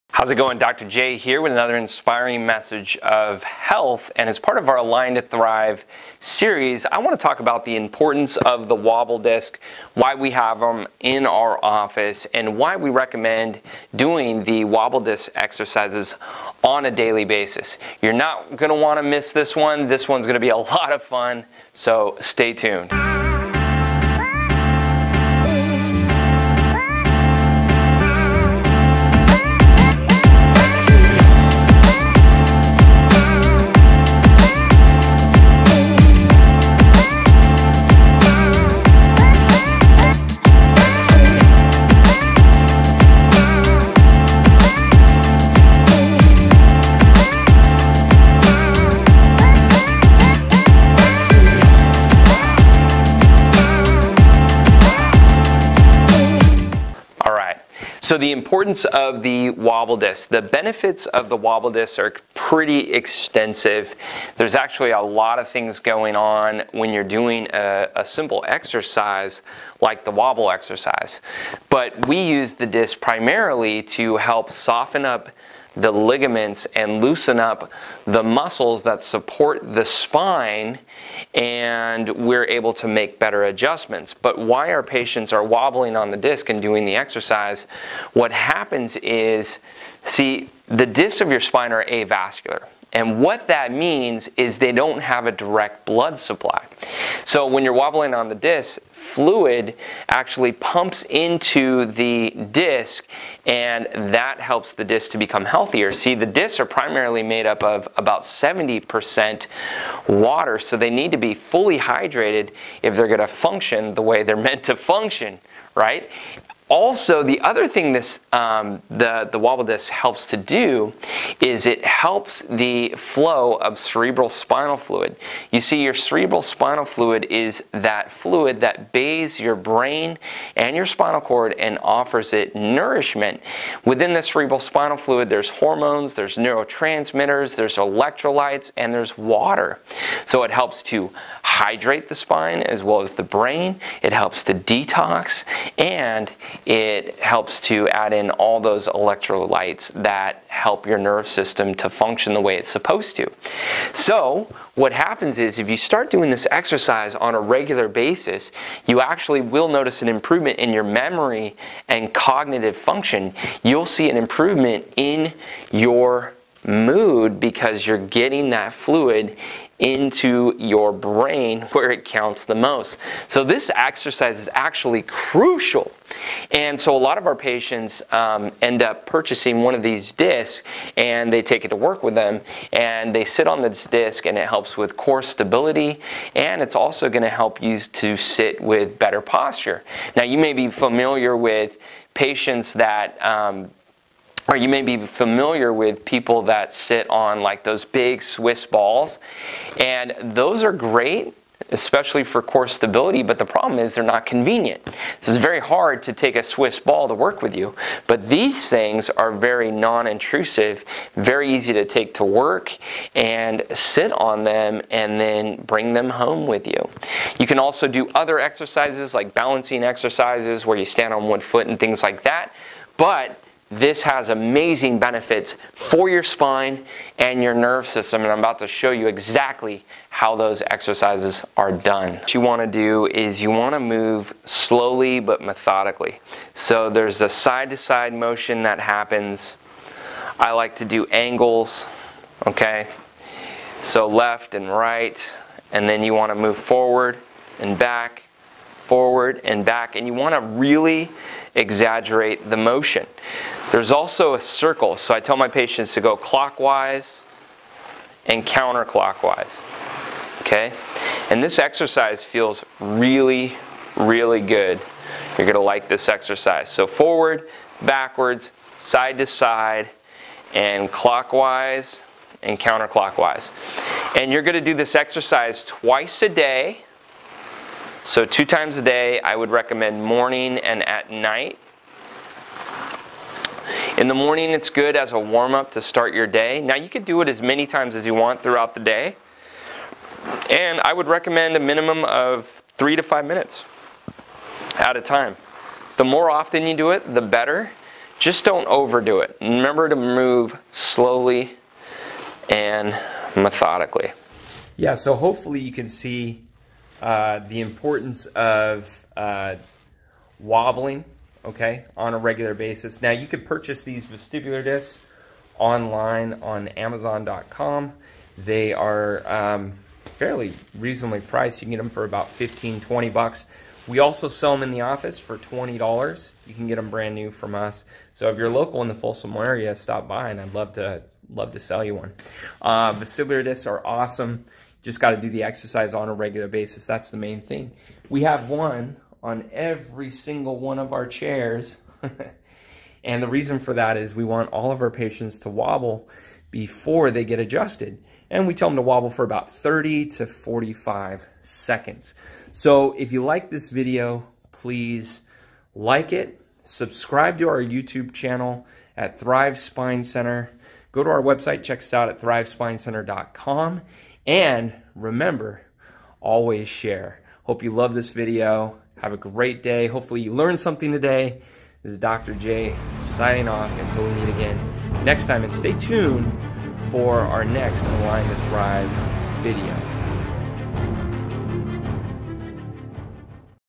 WOBBLE DISC EXERCISE (How to Create a Healthy Low Back)
Wobble Disc Exercise:

This exercise helps to pump fluid into the disc of your lower back. It also helps to circulate the cerebral spinal fluid that nourishes both your spinal cord and brain.

The CSF carries hormones, neurotransmitters, and electrolytes. This exercise helps to improve the motion of the spine as well as circulate the CSF. With better circulation of the CSF it can help to improve energy levels as well as your mood.

Do this exercise twice per day for 5-10 minutes. The disc also helps with posture and we recommend bringing it to work with you to sit on it throughout the day.